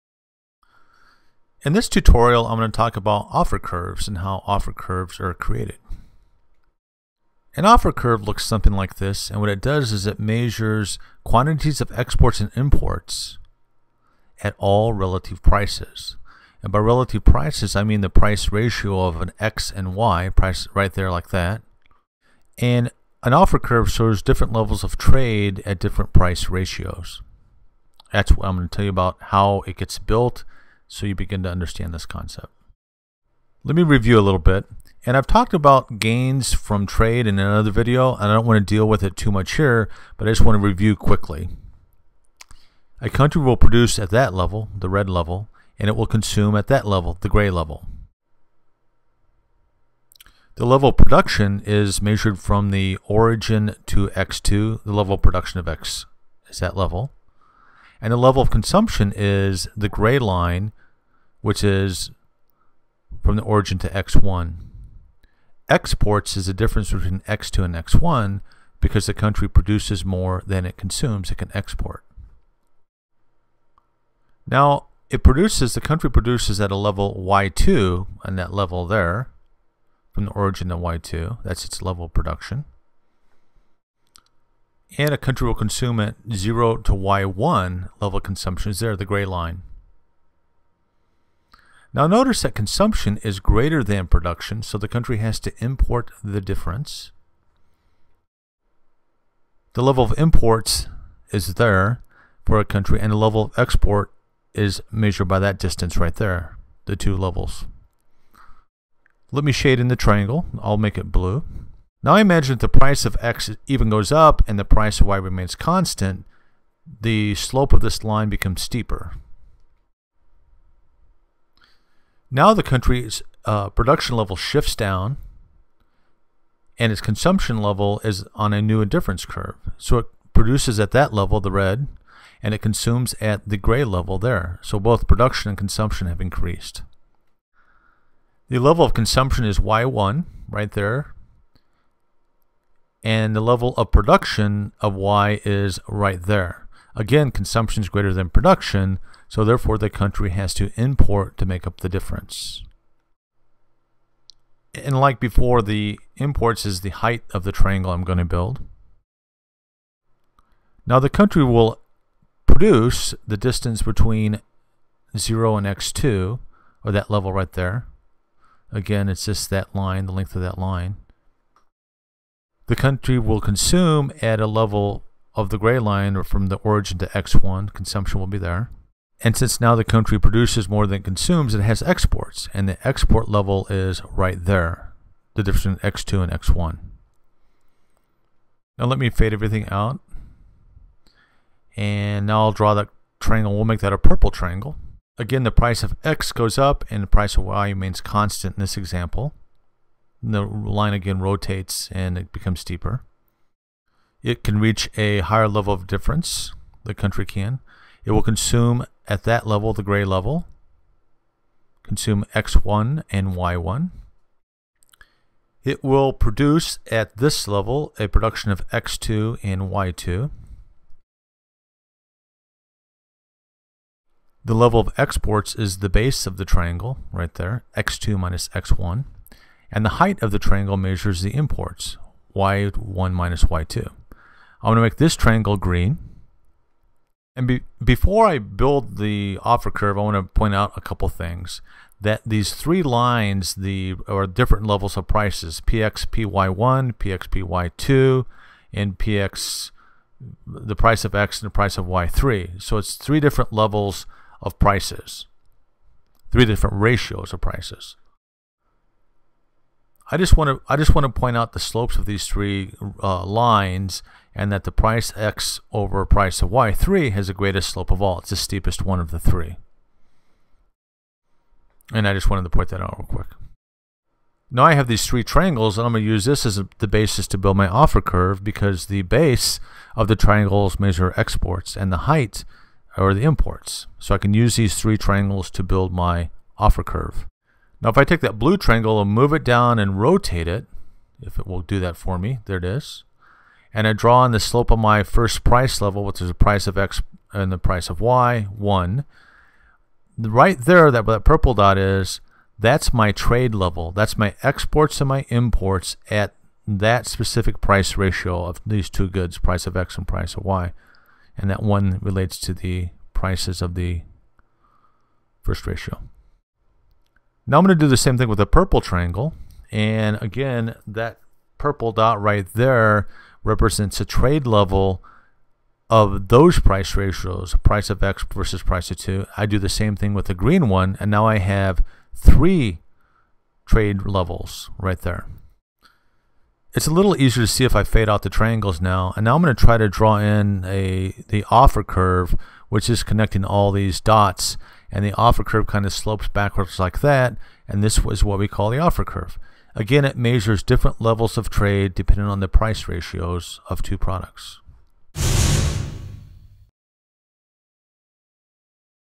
Offer Curves for International Economics Class
Tutorial on how to derive an offer curve..  Typically taught in a principles international economics or advance, microeconomics or managerial economics course.